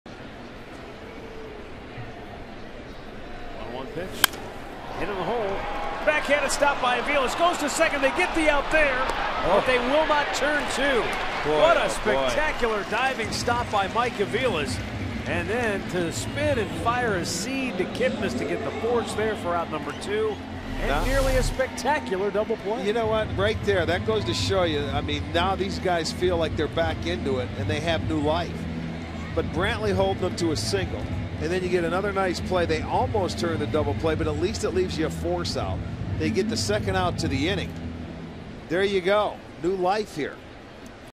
Aviles makes a nice stop in the seventh
6/8/13: Mike Aviles makes a nice diving stop on a grounder hit by Jhonny Peralta, then flips to second for the forceout in the seventh